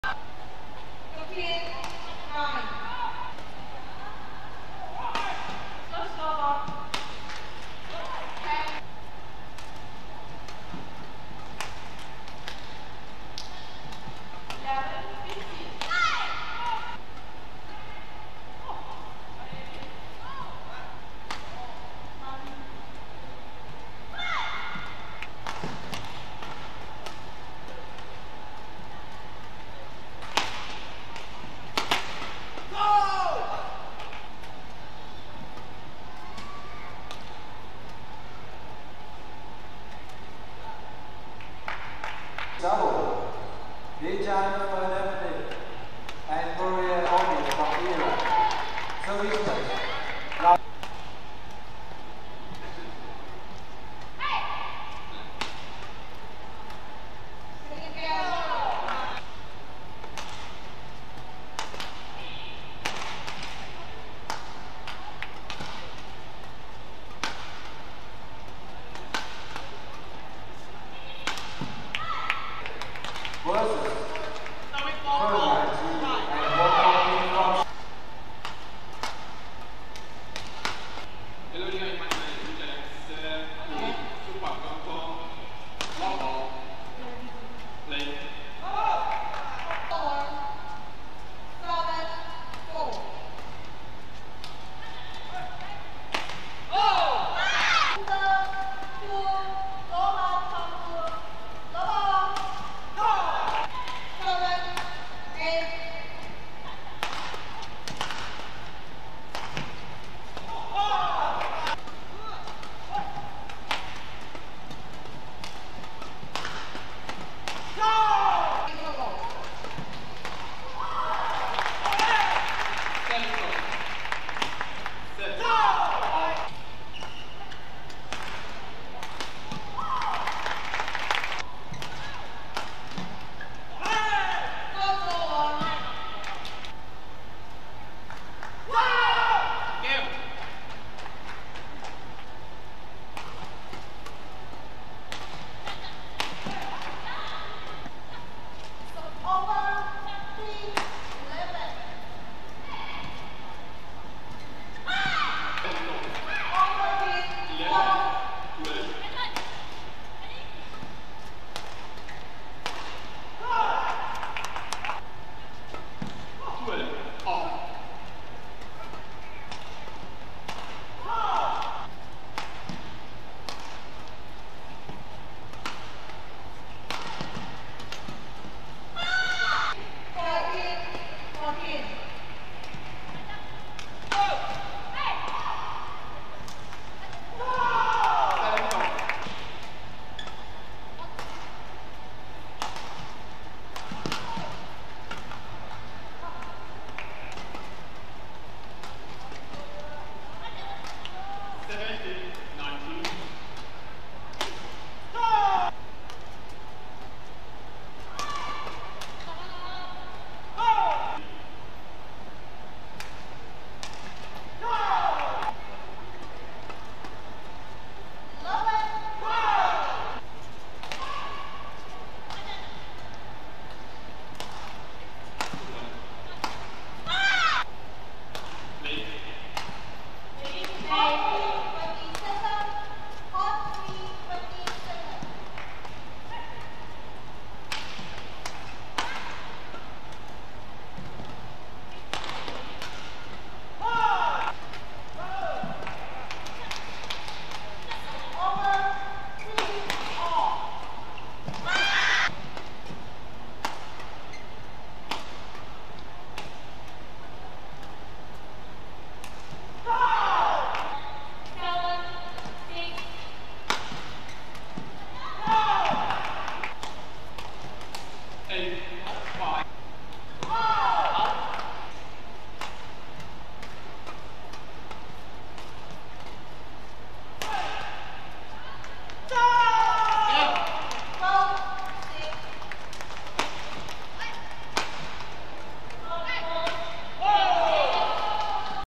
XD-R16 | CAHYONO/ISLAMI VS BHATNAGAR/CRASTO | PRINCESS SIRIVANNAVARI THAILAND MASTERS 2023
THAILAND MASTERS 2023
PRINCESS SIRIVANNAVARI THAILAND MASTERS 2023
Mixed's Doubles Round of 16
CAHYONO/ISLAMI VS BHATNAGAR/CRASTO
AKBAR BINTANG CAHYONO
MARSHEILLA GISCHA ISLAMI
HIROKI MIDORIKAWA
NATSU SAITO